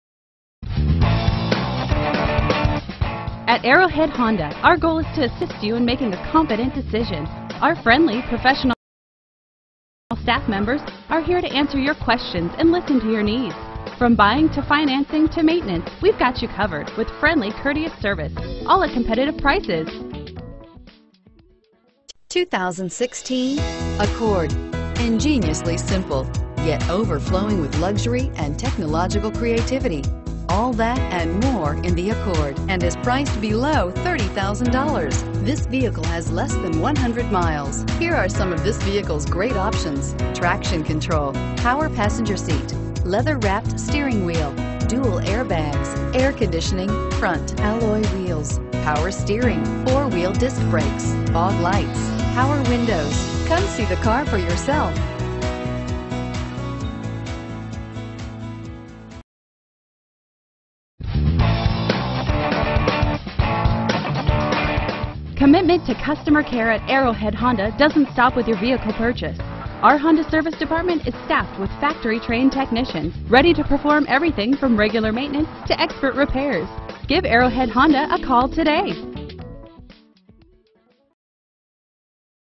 2016 Honda Accord Peoria AZ 15967
2016 Honda Accord EX-L

Arrowhead Honda
8380 West Bell Road

Black w/Leather-Trimmed Seats. At Arrowhead Honda, YOU'RE #1! Hey! Look right here! Is selling this Kona Coffee Metallic HondaAccord EX-L in Peoria. Arrowhead Honda is pumped up to offer this terrific 2016 Honda Accord. As sophisticated as you. Call, click, or stop by, we don't expect this vehicle to stay on the lot for long!